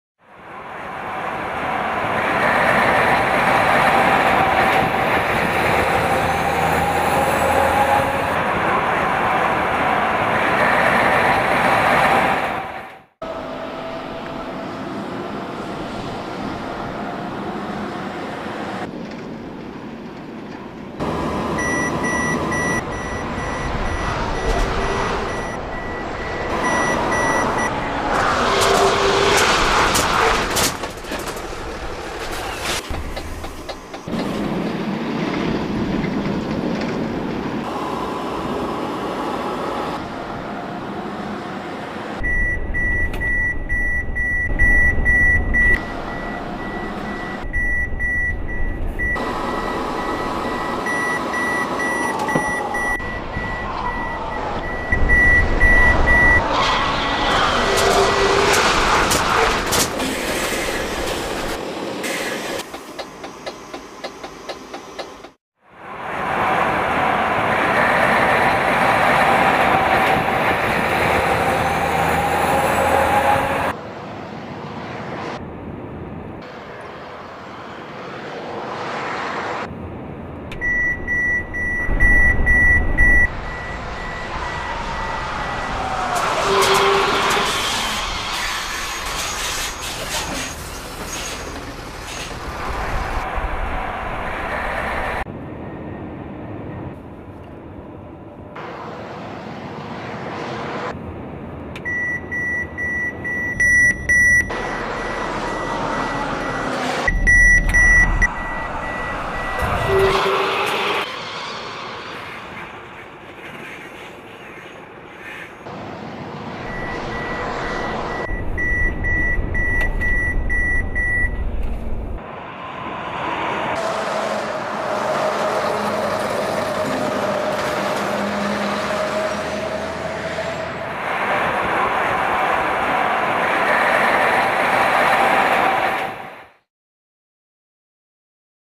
VOLVO TRUCKS BRAKE CHECKING
Advance braking system in trucks for avoid accidents